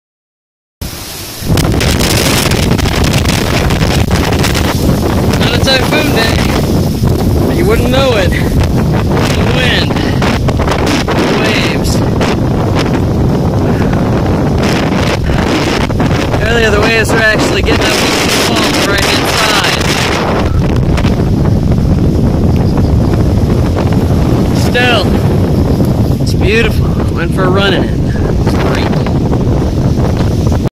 Unzen City, Nagasaki : Typhoon Winds in the Harbor
You may want to turn down the audio...

Our village in Unzen City, my family's hometown starting from 2013 while I am with the JET Programme - Nagasaki Prefecture, Japan.